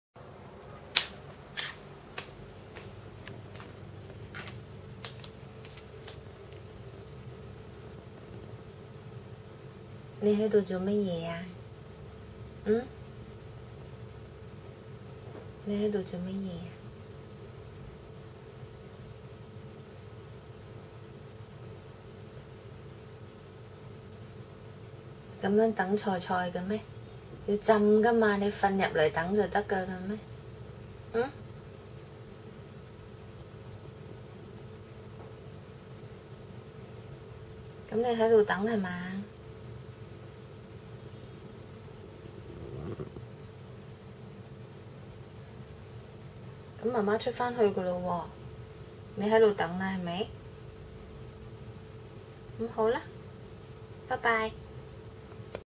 waiting for the veggie
Peter is waiting for the veggie in the kitchen.